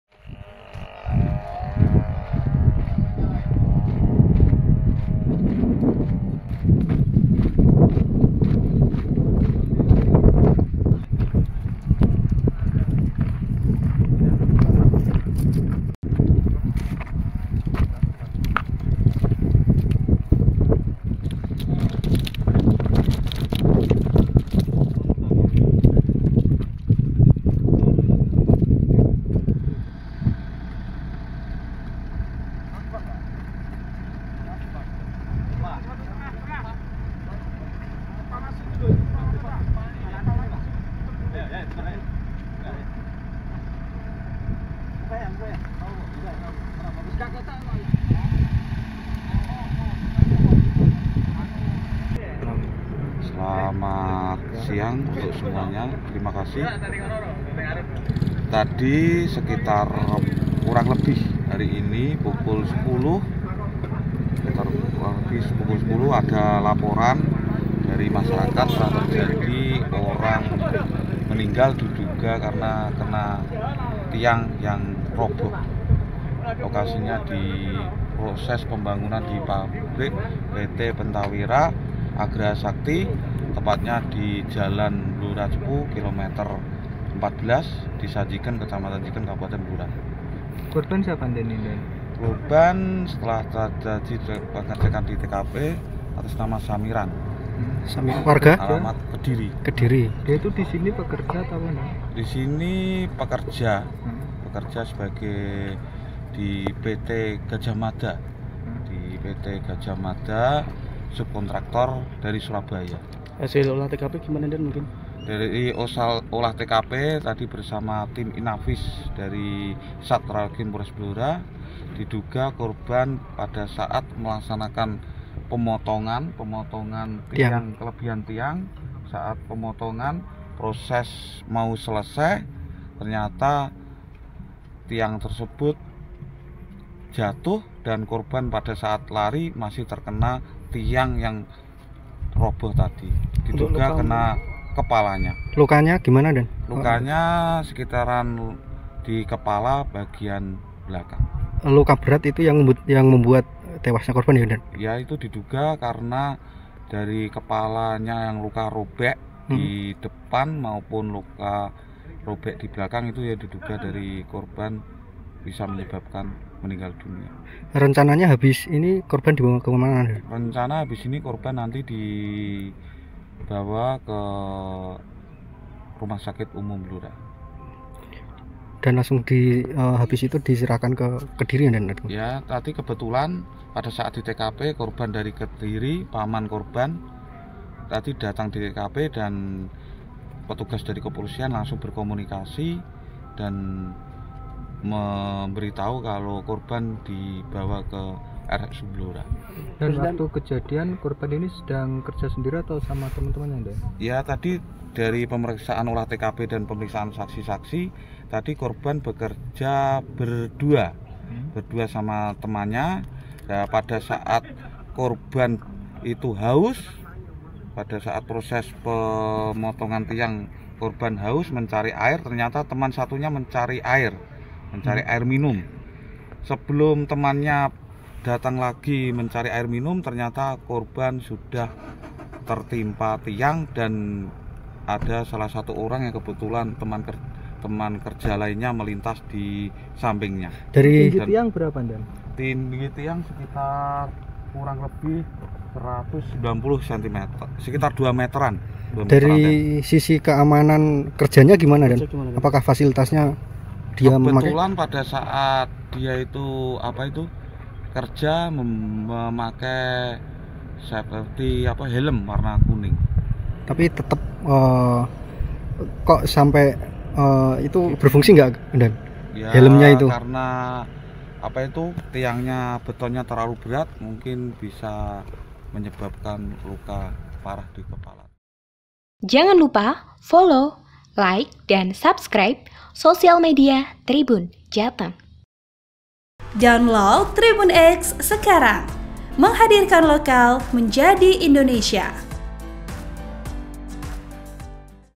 Kronologi Pekerja Bangunan Proyek Pembangunan Pabrik di Blora Meninggal Tertimpa Tiang Pancang
Nasib nahas menimpa pekerja bangunan, Samiran (50), yang tewas tertimpa tiang pancang saat proses pengerjaan proyek pembangunan di PT Pentawira Agraha Sakti, Rabu 26 Juni sekira pukul 10.10.

Korban bernama Samiran (50), warga Kediri, Jawa Timur.

Status korban merupakan pekerja bangunan di PT Gajahmada Putera Jaya, subkontraktor dari Surabaya.

Penyebab korban meninggal lantaran terdapat luka berat di bagian kepala korban.

Editor Video: edr